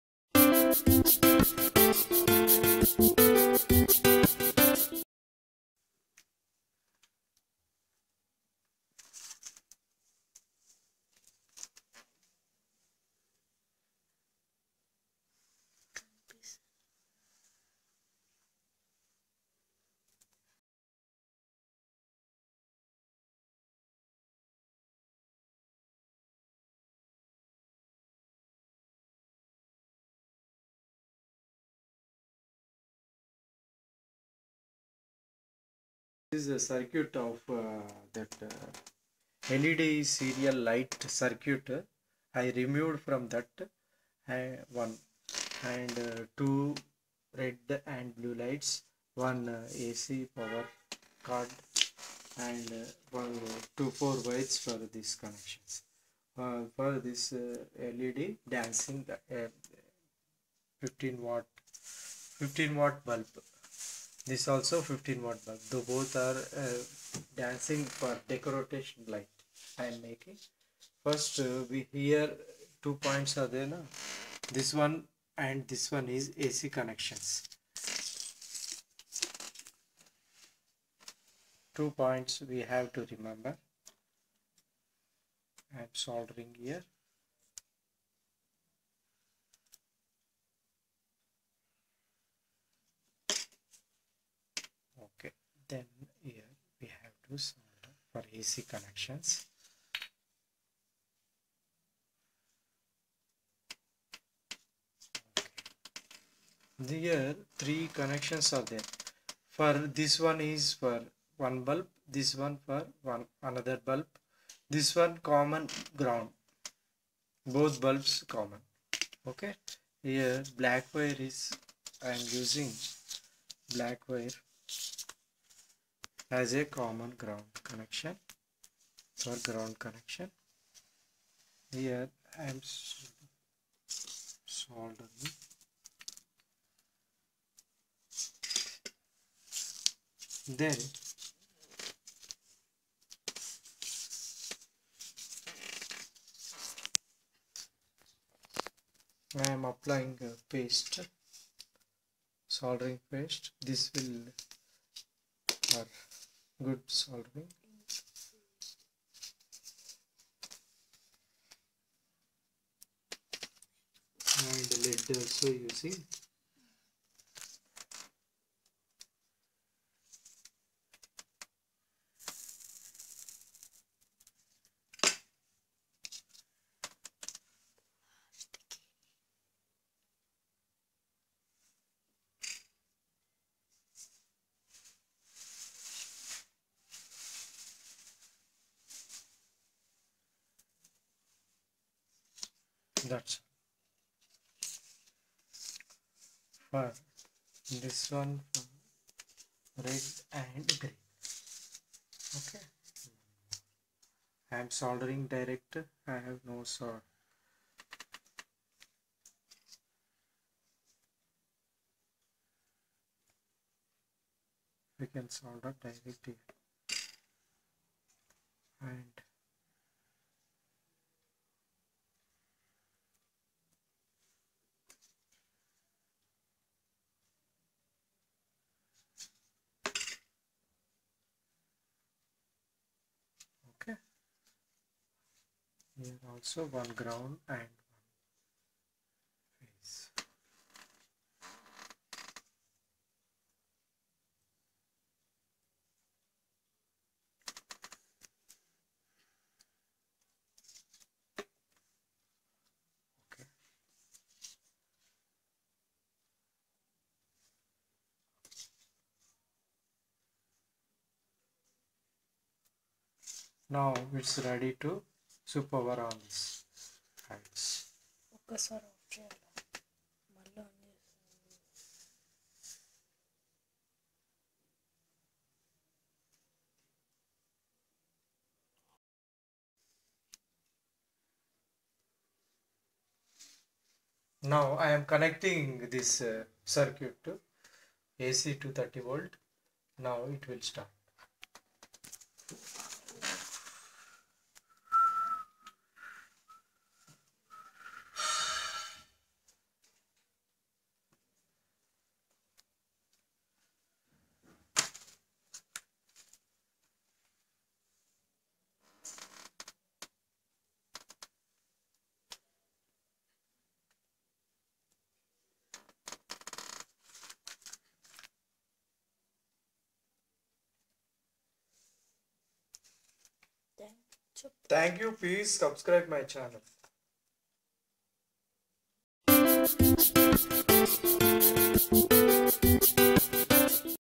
MAKE YOUR OWN DANCING LIGHTS BULBS AC 230V or 120v
simple making dancing bulbs using  15w bulbs , removed led circuit and connected two colored bulbs
ac dual lamp blinker using home decorated led series  circuit